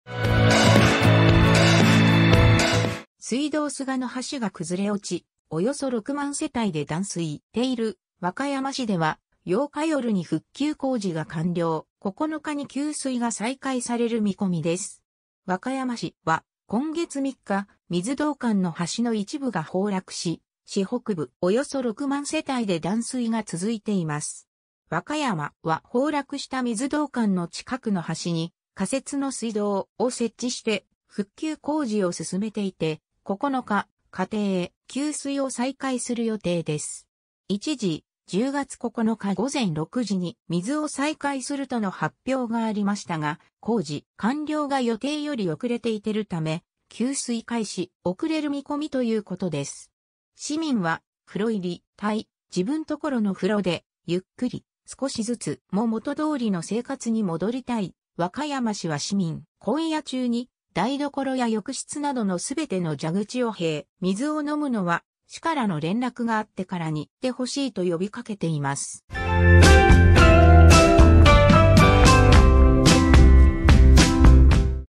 ニュース 北海道 |約６万世帯断水の和歌山市 仮設の水道管で９日給水再開へ| 北海道NEWS
ニュース速報
水道菅の橋が崩れ落ち、およそ6万世帯で断水している和歌山市では、8日夜に復旧工事が完了し、9日に給水が再開される見込みです。 和歌山市では今月3日、水道管の橋の一部が崩落し、市北部のおよそ6万世帯で断水が続いています。 和歌山市は崩落した水道管の近くの橋に仮設の水道管を設置して復旧工事を進めていて、9日、家庭への給水を再開する予定です。 一時、10月9日午前6時に給水を再開するとの発表がありました
© Copyright by 北海道 NEWS   ☞ “Do not Reup”